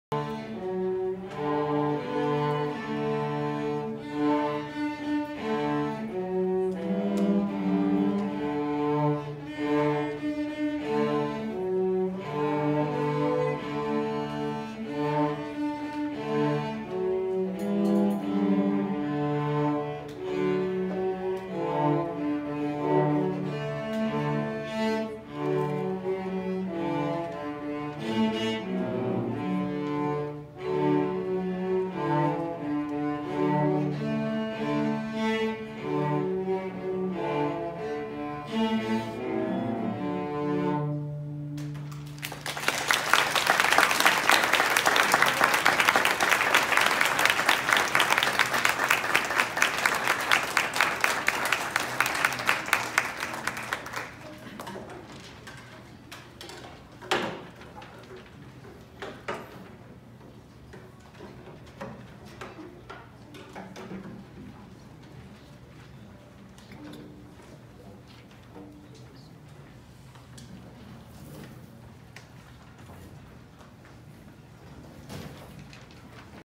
Jasmine's cello Performance
Jasmine performed with her peers at the Westminster School.